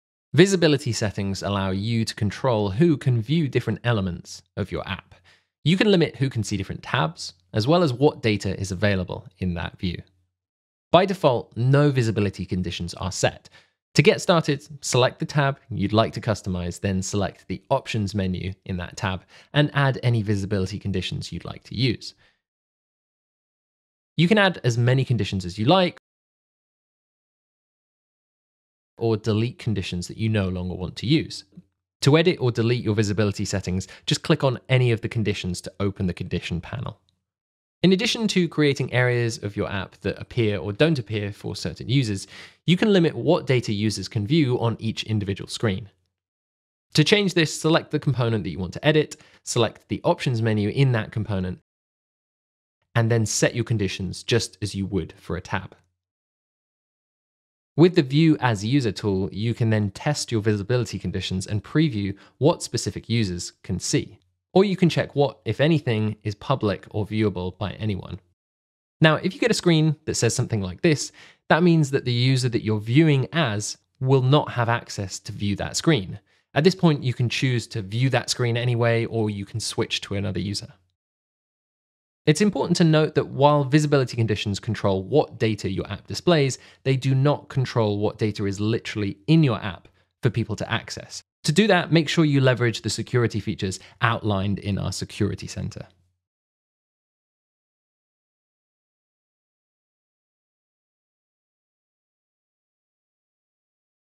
Visibility Conditions | Glide App Tutorial
In this quick tip video, we will dive into Glide's powerful Visibility features, allowing you to take control of who can view specific elements of your app. With the ability to set visibility conditions, you can easily limit access to certain tabs and data within your chosen view. Whether you need to restrict certain information to specific users or create customized experiences, this feature has got you covered.

With the flexibility to edit or delete visibility conditions as per your preference, you have full control over how your app is perceived by different users. Additionally, you can even limit data visibility on individual screens, ensuring that each user only sees the information they need. Using the 'view as user' tool, you can test and preview screen displays based on specific visibility conditions, providing you with peace of mind and confidence in your app's performance.

It's important to note that while these visibility conditions control what data your app displays, they do not regulate data accessibility within the app itself. To manage data access, we encourage you to make use of the security features available in the Security Center. This will further enhance the protection and privacy of your app's data.